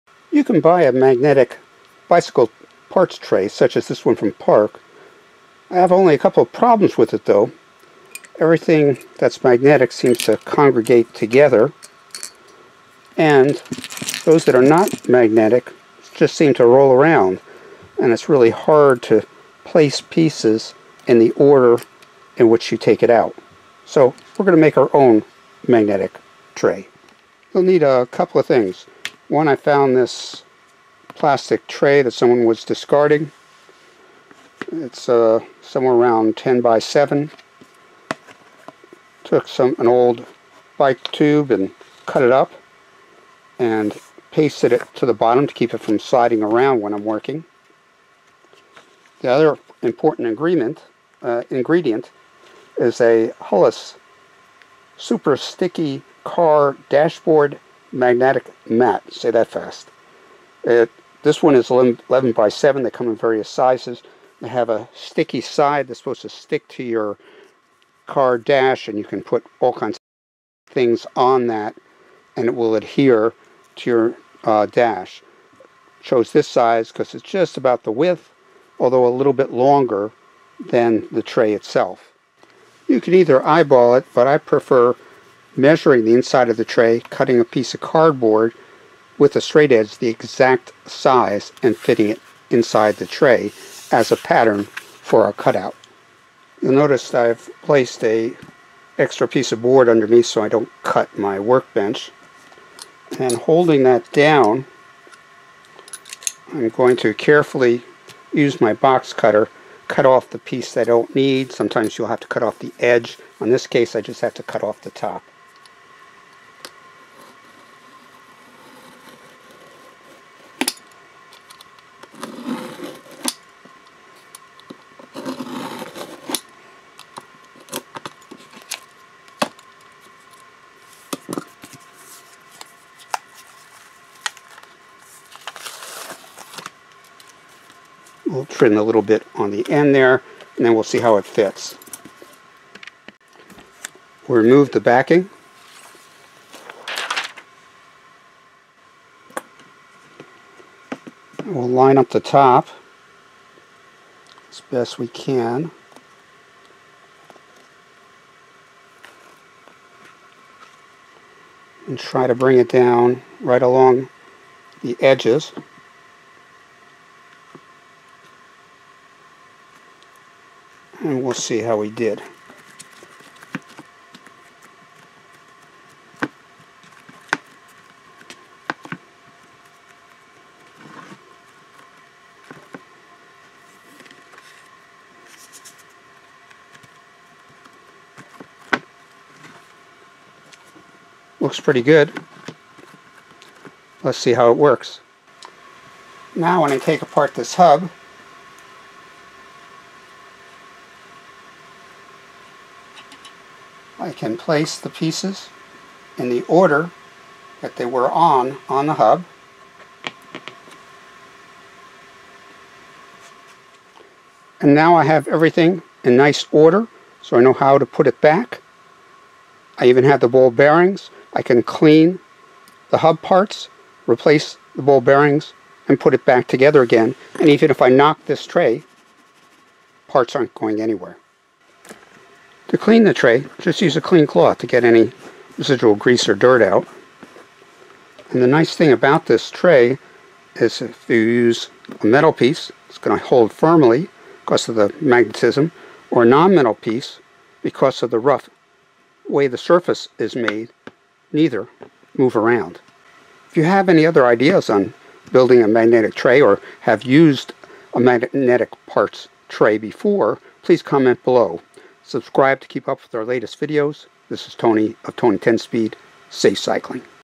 DIY: Homemade Magnetic Parts Tray
You can buy a magnetic parts tray such as this one from Park Tool for bicycle parts. The only problems I have with using such trays are:
1. Items tend to move together in the center of the magnet.
2. Nonmagnetic items just roll around on the tray.
Ideally, I would like a tray I can lay out my part in order that I disassemble them such as when working on such things as hubs. I'd like to have a magnetic surface distributed across the tray so they won't be pulled together and also have a surface that will keep nonmagnetic parts and small ball bearings from rolling around with such gadgets.

So, here's how to made your own inexpensive magnet parts tray (under $10). For this project, I found an old plastic tray so one was discarding. Put some rubber strips on the bottom to keep them from rolling around (you can use an old computer mouse pad or even cut up an old mountain bike tube).

Then I purchased a Hulless 11 x 7 Super Sticky Car Dashboard Magnetic Mat from Amazon for $8. from Amazon:

Cut it to size. Took off the backing and stuck it on to my try.

Now, when I overhaul a hub (or am removing screws, nuts and bolts), I can lay out the pieces nicely on the magnetic tray and nothing rolls around, even as I move the tray. An excellent parts holder as well as small tool holder for bike maintenance and bike repair.